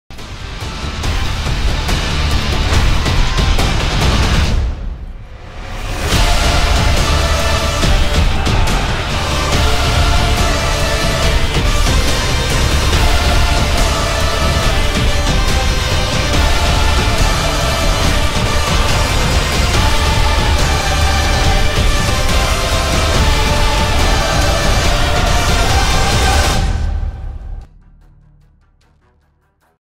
Intro Mix-Dla Mnie |#2
To jest drugie intro tym razem się  bardziej przyłożyłem i teraz mi napiszcie które jest lepsze;D

Oceniaj
Komentuj